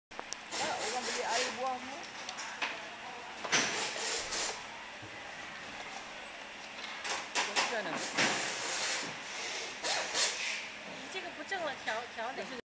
Semi-auto Cup Sealing Machine
Manual bubble tea or soymilk cup sealer is to seal drink cups for the take-away drinks.Convenient and affordable.
Pls contact Now！